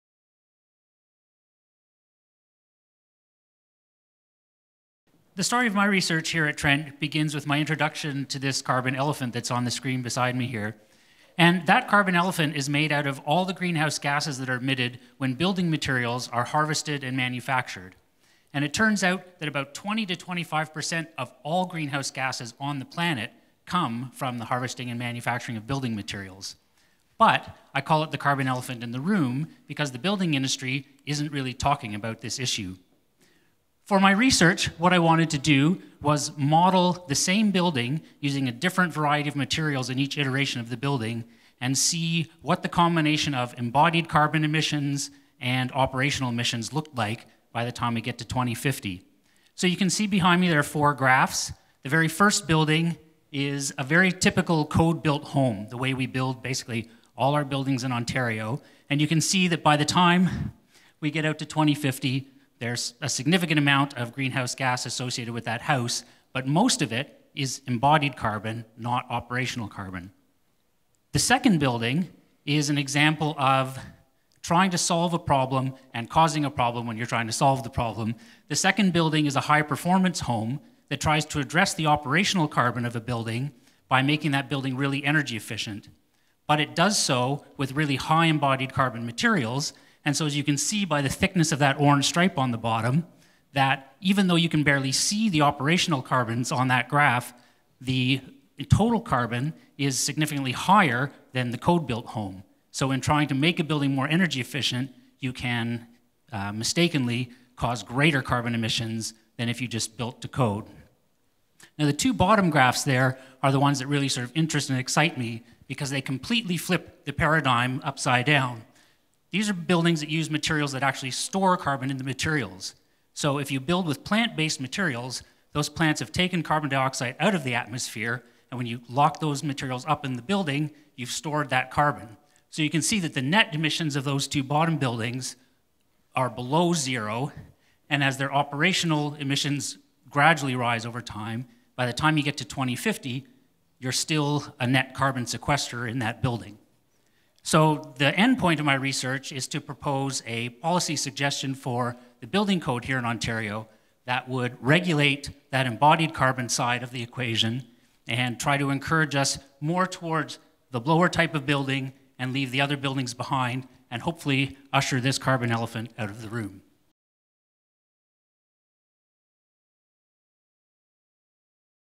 Trent University 3MT 2018: President's First Prize Winner, Chris Magwood
Chris Magwood, a Masters student in Sustainability Studies at Trent University, won the President's First Prize at Trent's 2018 Three Minute Thesis Competition. His talk is titled, "GHG Drawdown and Carbon Storage in Buildings."